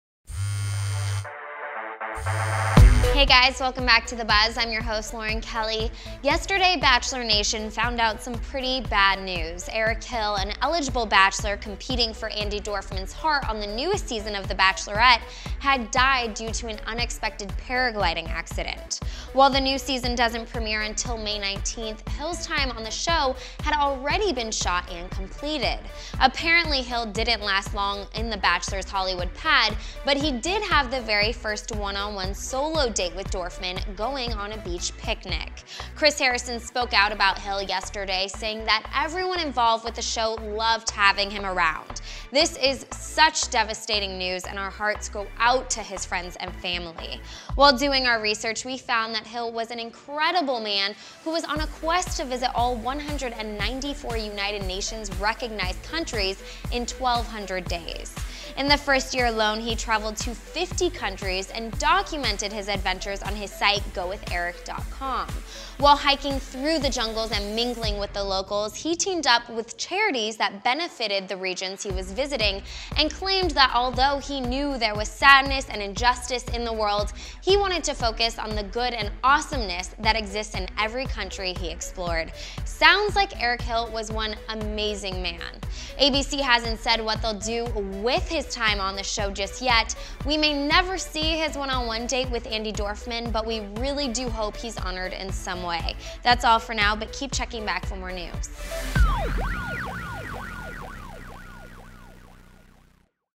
Details on Eric Hill and His Short Stint on The Bachelorette - The Buzz
Bachelorette contestant Eric Hill dies in a paragliding accident.